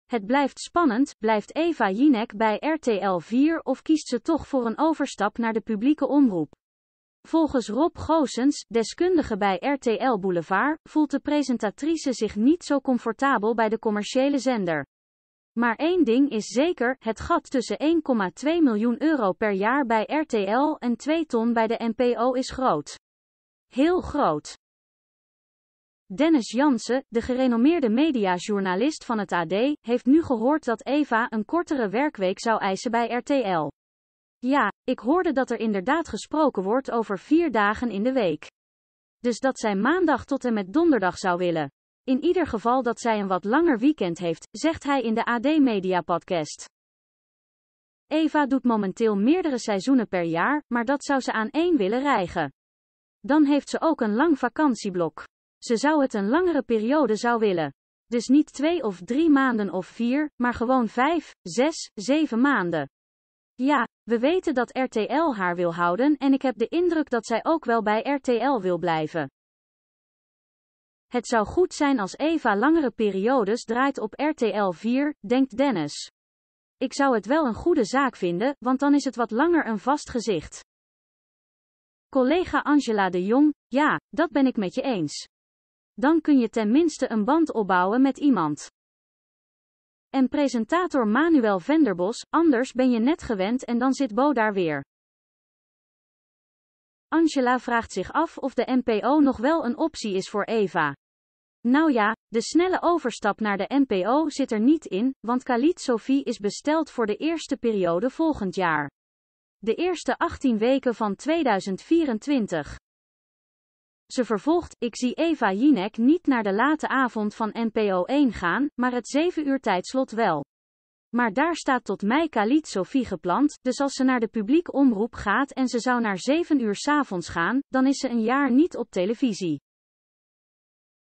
Eva Jinek wil kortere werkweken bij RTL: ‘Word lekker huisvrouw!’
Eva Jinek wil volgens het AD kortere werkweken als ze bij RTL blijft, met uitzendingen van maandag tot en met donderdag. Critici worden er moe van: “Ga lekker huisvrouw worden!”